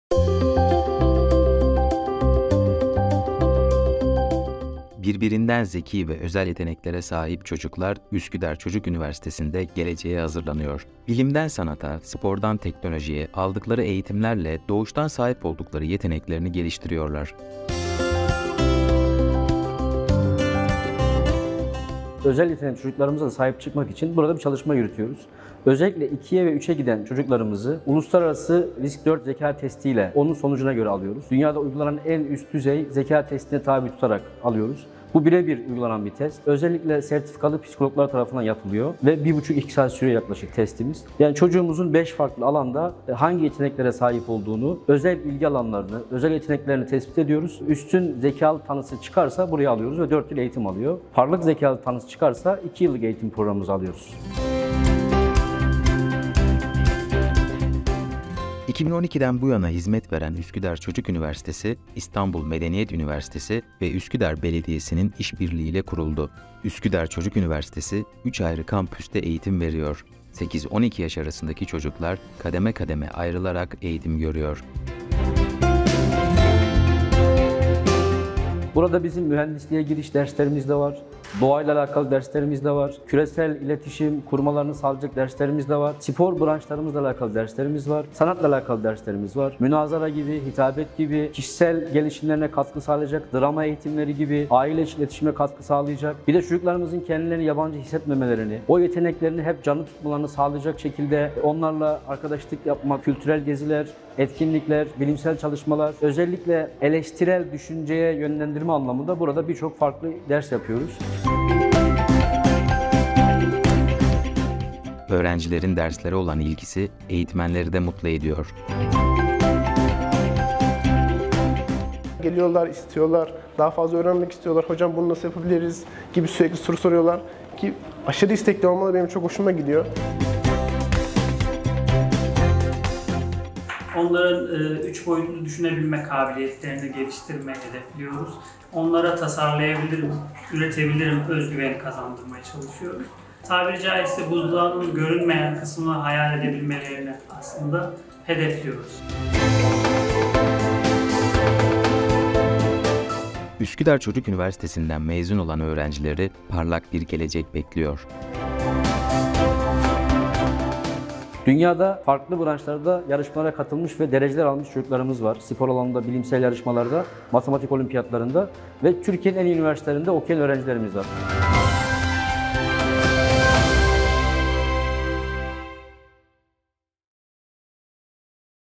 Türkiye’nin ilk ve tek YÖK onaylı çocuk üniversitesi Üsküdar’da
Hepsi birbirinden özel yetenekli öğrenciler Üsküdar Çocuk Üniversitesi çatısı altında bir arada eğitim alıyor. Bilimden sanata, spordan teknolojiye pek çok alanda eğitim alıyorlar. Belki de geleceğin bilim insanları, üstün yetenekli sanatçıları hatta sporcuları burada yetişiyor.